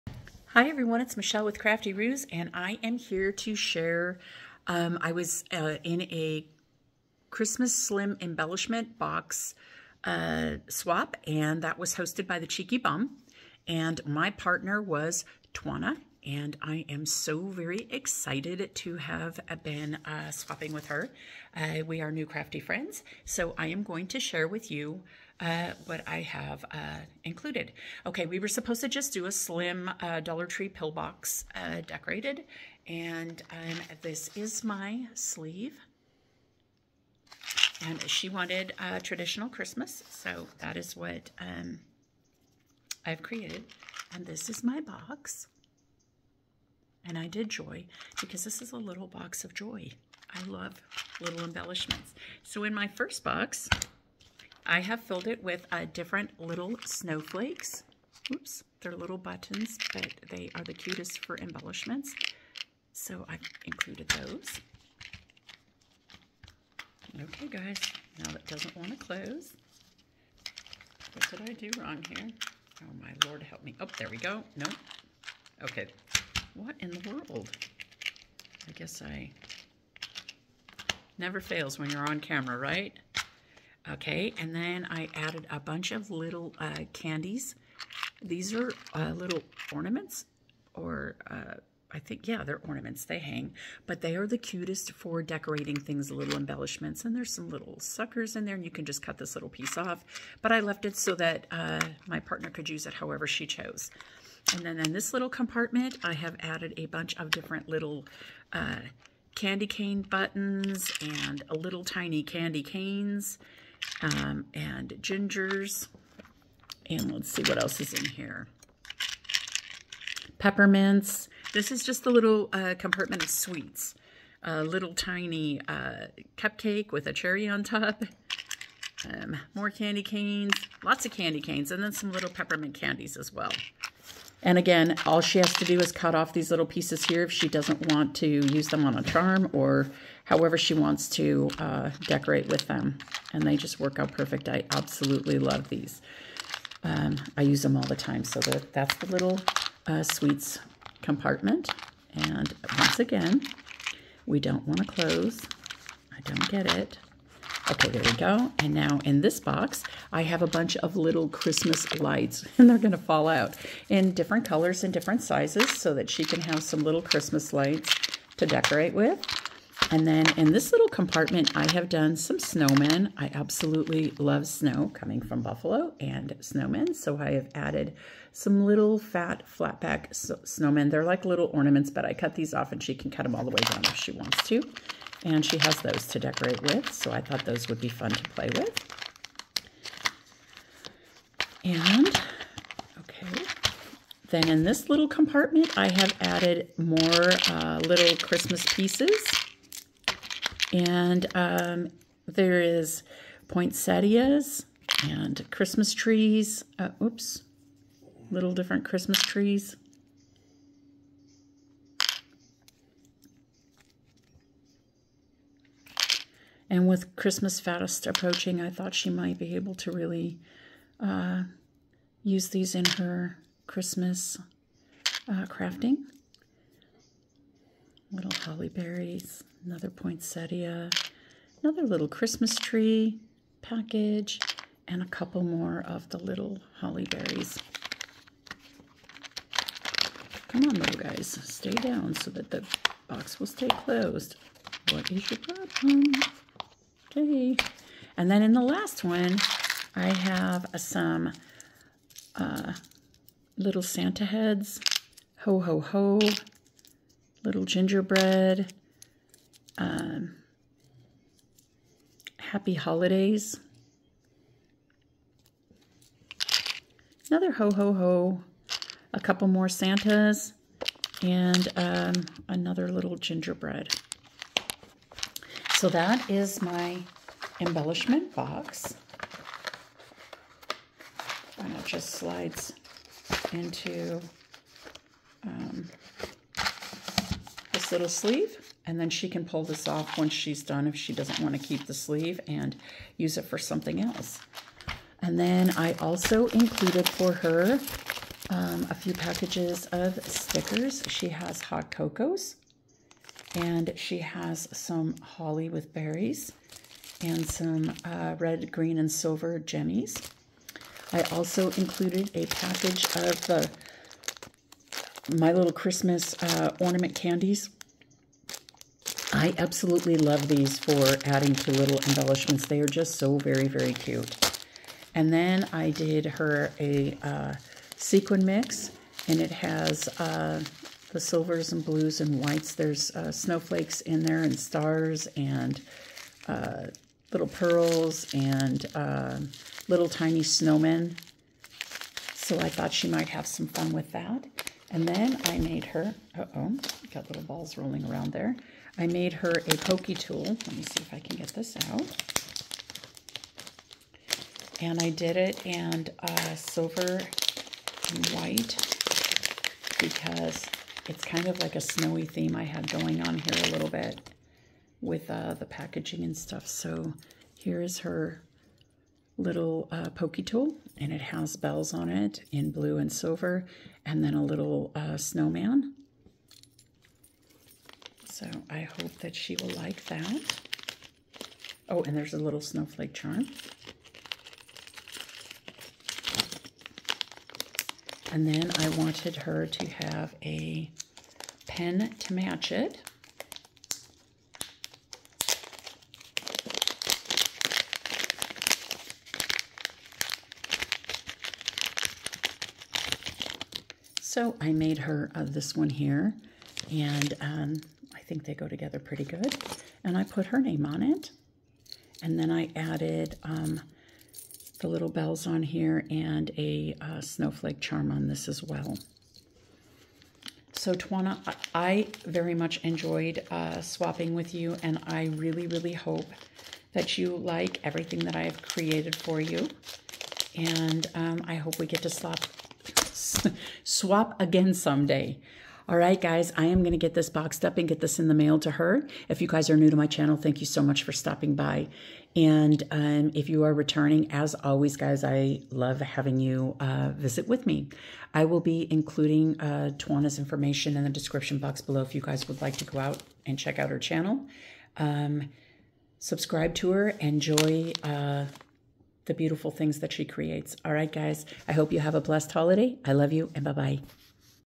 What I created for my partner in The Christmas slim embellishment swap hosted by @Thecheekybumm!
This was a dollartree slim pill box embellishment swap.
My partner is Twyanna Tate.